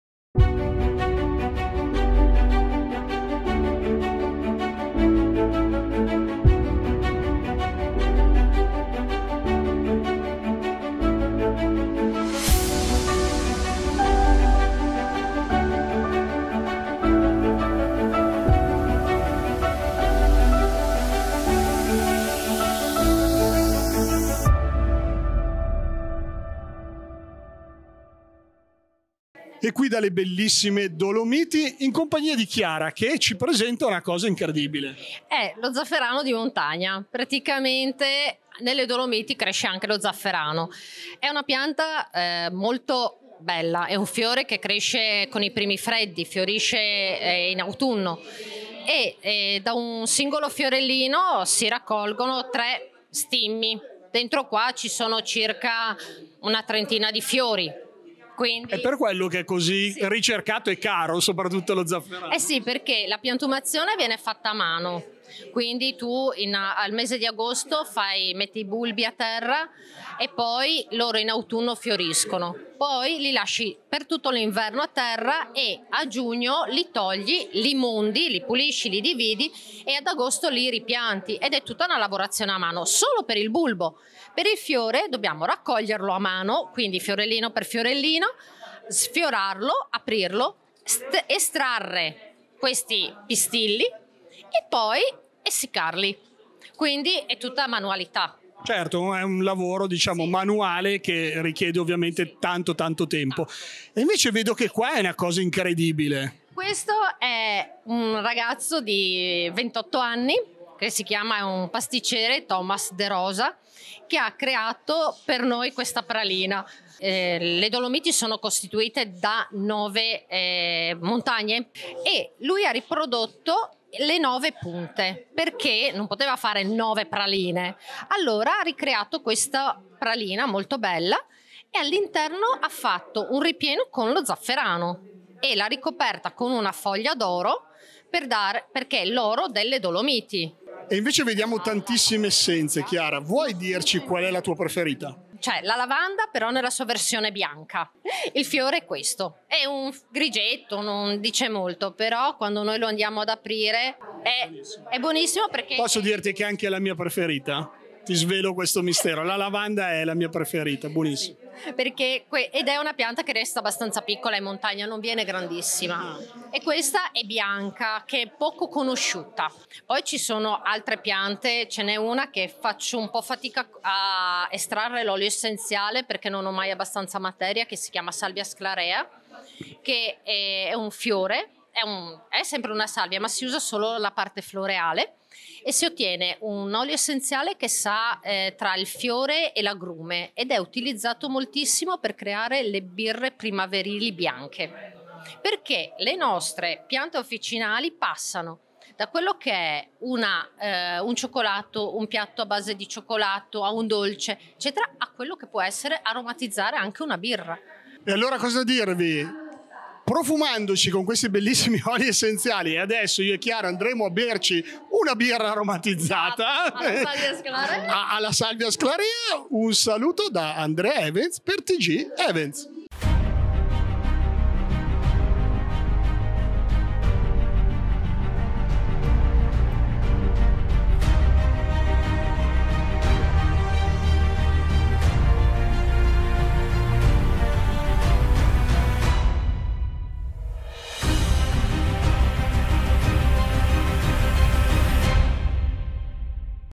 Zafferano di Montagna (Dolomiti) - TGEvents Television
Tgevents Television in collaborazione con la A&A Studios Presentano:

Azienda Agricola Saliet,
nasce nel 2016 da una radicale scelta di vita dettata dalla passione per la montagna e dal fascino per il mondo delle piante officinali.
L’uso delle erbe è da sempre in equilibrio tra magia e scienza, accompagna l’uomo fin dall’antichità, trova il suo massimo splendore nei secoli bui del medioevo e ci accompagna fino ai giorni nostri.
Queste forze hanno dato il via ad un progetto che si è tradotto in realtà attraverso l’avvio di un’azienda agricola nel cuore delle Dolomiti friulane all’interno del piccolo comune montano di Claut (PN)

Un tocco di esotico nelle Dolomiti. È “Zafferano Dolomiti”, il progetto che porta l’oro rosso a Belluno, in Veneto. La pianta dello zafferano ama le basse temperature, anche sotto zero, è adatta perciò alle coltivazioni di altitudine, per questo è venuta l’idea di portare la pregiata coltivazione in territorio bellunese, prendendo spunto da una realtà in provincia di Bergamo. Si hanno tracce dello zafferano in Veneto già dai tempi della Repubblica di Venezia, quando i bulbi venivano portati dai commercianti di ritorno dai viaggi in Medio Oriente. Il sogno di Zafferano Dolomiti, che per ora coinvolge circa 60 produttori tra Valbelluna, Feltrino, Agordino, Val di Zoldo e Cadore, è quello di unire tutti i produttori della provincia in un’unica realtà.

Title:                  Zafferano di Montagna
Event:                TGEvents Puntata 631
Location:           Azienda Agricola Saliet - Claut PN
Canali TV          BOM Channel 68 Martedì ore 20,00
Date:                  26 Febbraio 2023
Presenter:         Andrea  Events
Video Edit:        Beppe Puglisi
Photo&Video:   Damiano Colle,
Powered:           B&D Production e A&A Studios Milano

Special thanks: Marco Dottore, Luca Galtieri,

A&A Studios
Via B. Maderna, 2
Milano